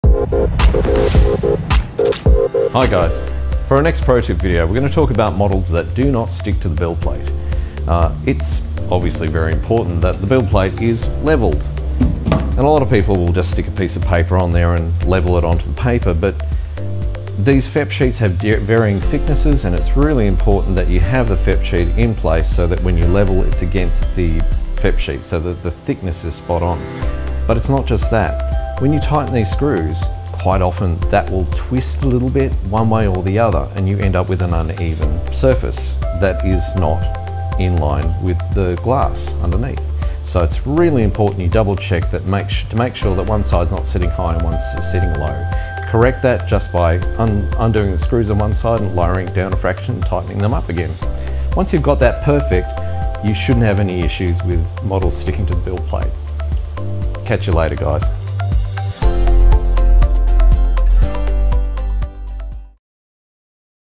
Monocure 3D ProTips Bytes: Why are your 3D resin prints might be falling off the build plate.
In our latest ProTips 'BYTES,' John explains one way to solve the problem of your 3D printed resin models falling off the build plate.

Thanks for the excellent tip John!